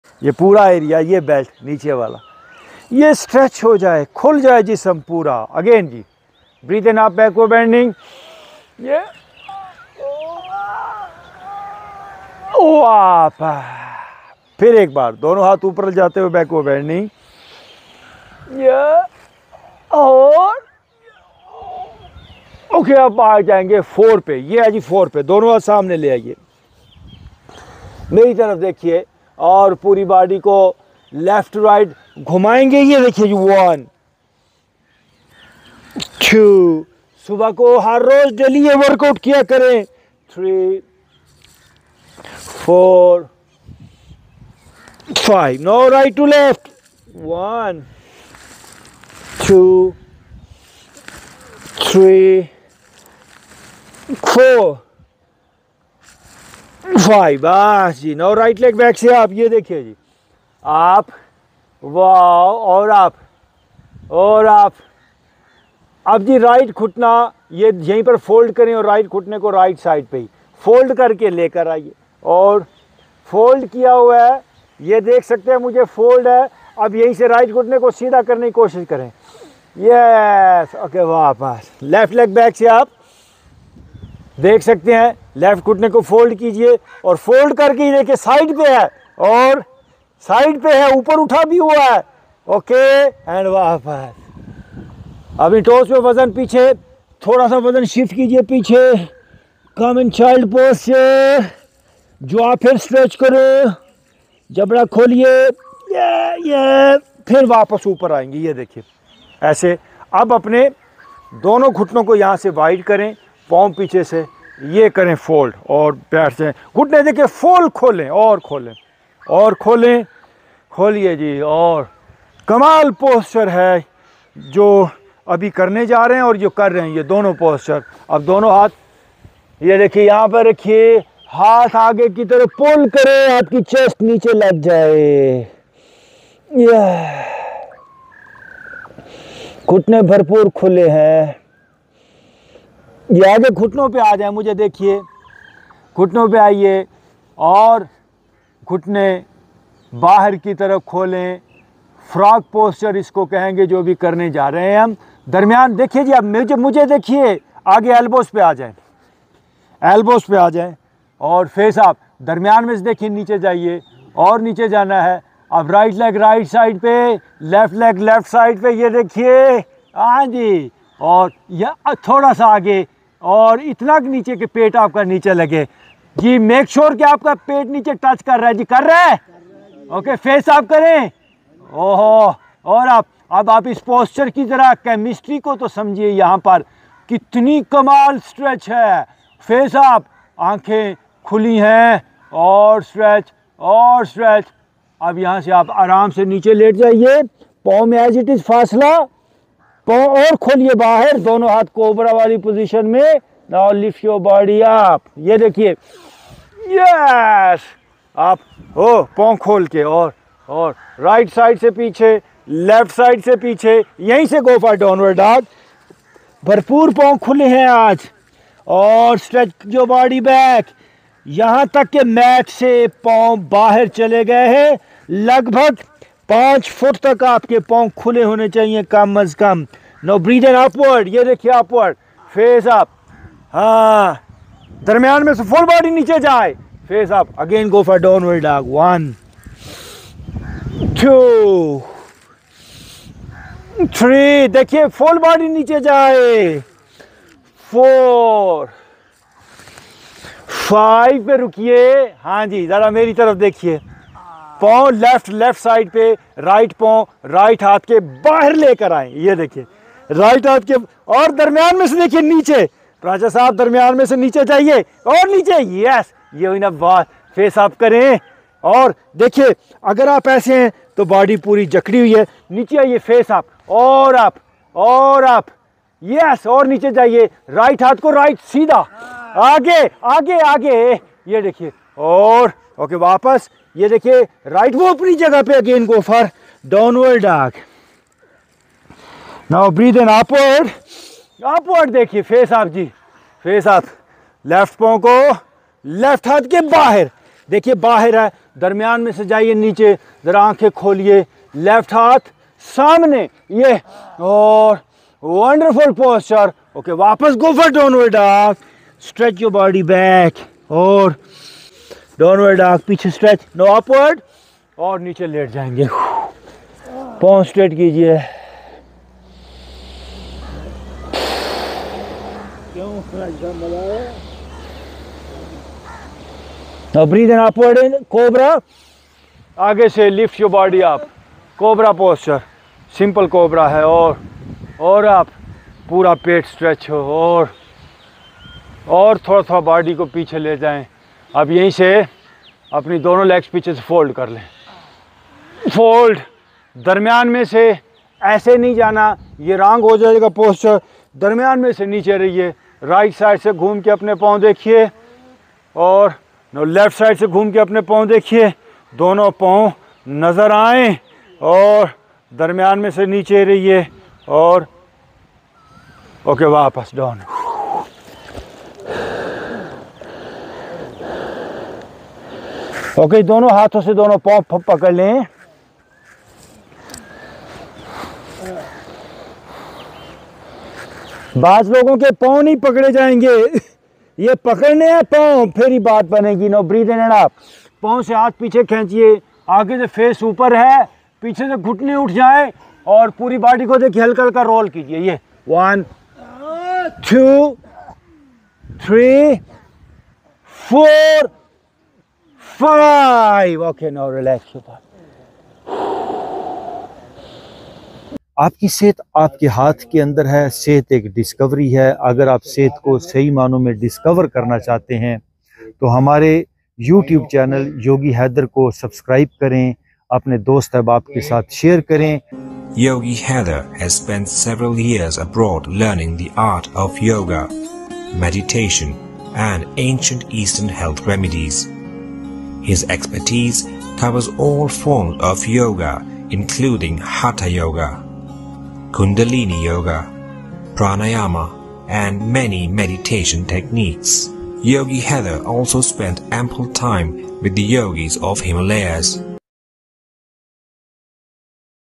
Yoga for Prostate Problems | Fix Your Prostate
Hoe to Treat Prostrate
Yoga sy Prostate ka elaj
Yoga for prostate and all reproductive Problems
Yoga & Meditation is a way to achieve self Insight, Mind & Body Harmony & absolute cure for all Physical & Psychological Disorders.
We can improve our heart and lungs functioning with these simple exercises .